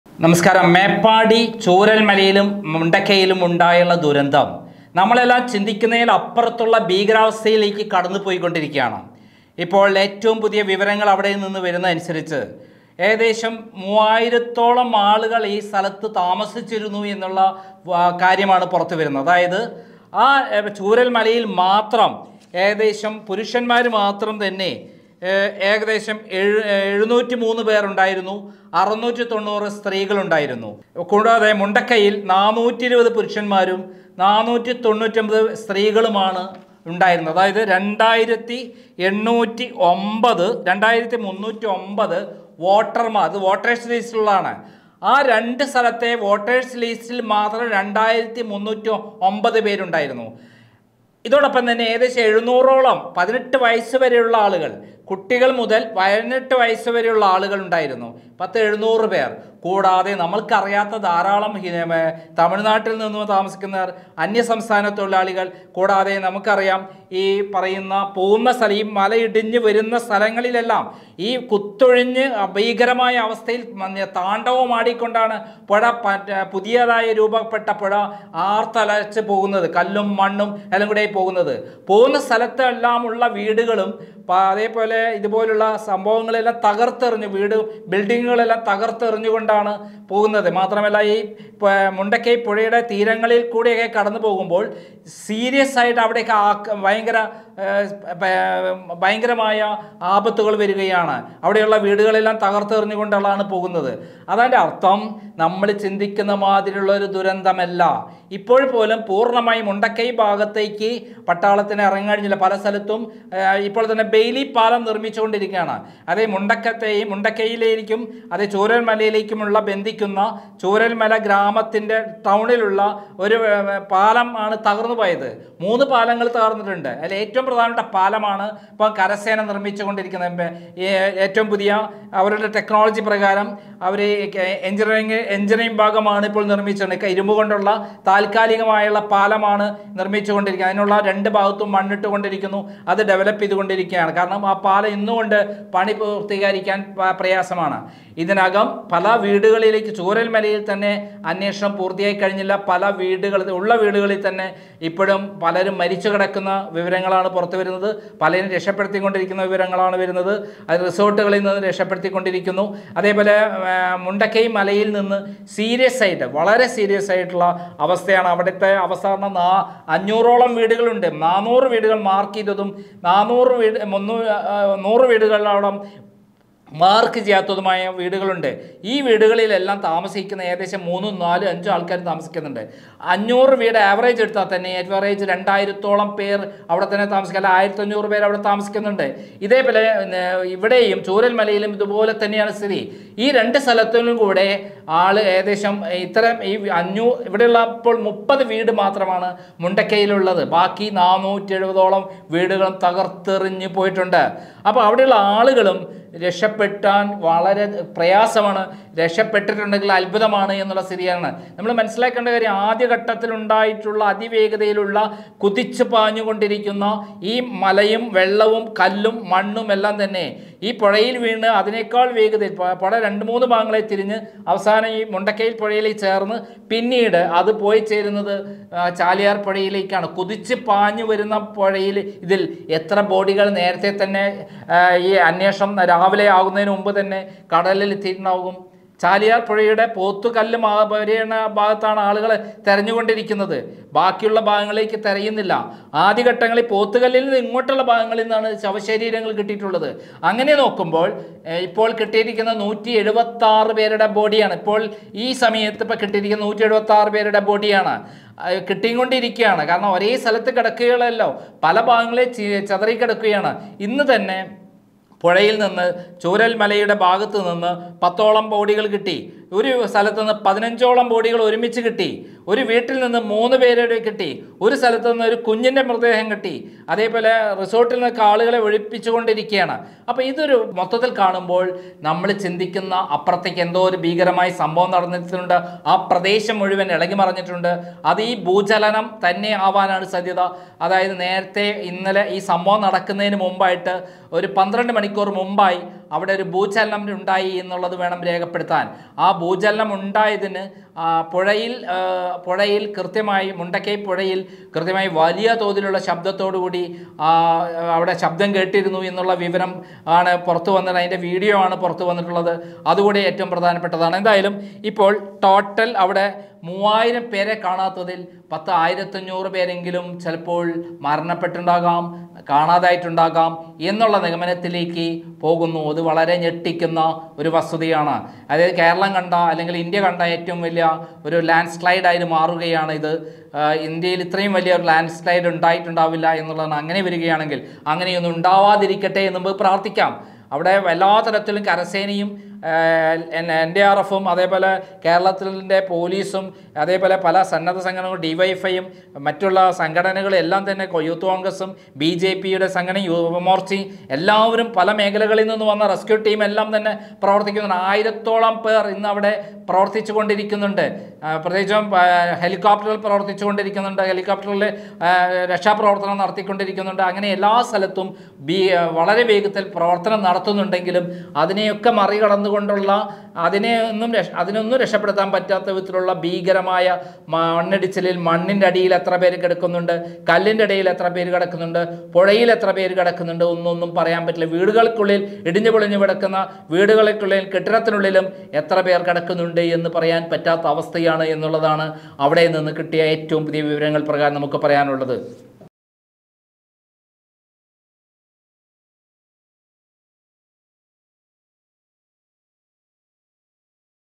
500 വീടുകൾ ഒലിച്ചുപോയി മരണം 1500 കടക്കുമോ?? | Meppadi Landslide
500 വീടുകൾ ഒലിച്ചുപോയി മരണം 1500 കടക്കുമോ?? | Meppadi Landslide |